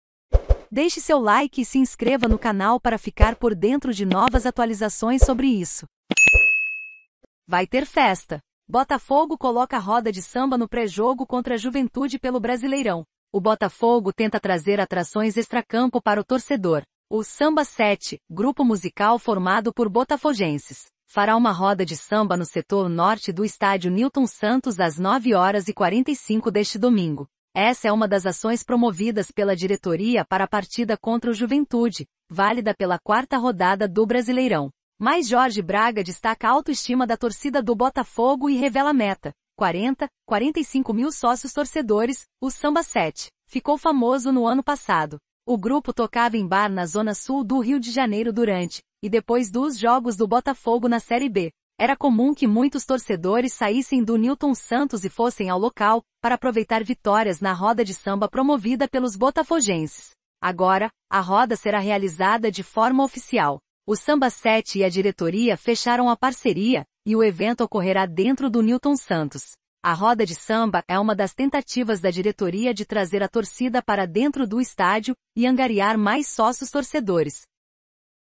VAI TER FESTA! BOTAFOGO COLOCA RODA DE SAMBA NO PRÉ JOGO CONTRA JUVENTUDE PELO...
Vai ter festa! Botafogo coloca roda de samba no pré jogo contra Juventude pelo Brasileirão

O Botafogo tenta trazer atrações extracampo para o torcedor. O “Samba 7”. grupo musical formado por botafoguenses. fará uma roda de samba no Setor Norte do Estádio Nilton Santos às 9h45 deste domingo. Essa é uma das ações promovidas pela diretoria para a partida contra o Juventude. válida pela 4ª rodada do Brasileirão. + Jorge Braga destaca autoestima da torcida do Botafogo e revela meta: ’40. 45 mil sócios torcedores’ O “Samba 7” ficou famoso no ano passado. O grupo tocava em bar na Zona Sul do Rio de Janeiro durante e depois dos jogos do Botafogo na Série B. Era comum que muitos torcedores saíssem do Nilton Santos e fossem ao local para aproveitar vitórias na roda de samba promovida pelos botafoguenses. Agora. a roda será realizada de “forma oficial”. O Samba 7 e a diretoria fecharam a parceria e o evento ocorrerá dentro do Nilton Santos. A roda de samba é uma das tentativas da diretoria de trazer a torcida para dentro do estádio e angariar mais sócios torcedores.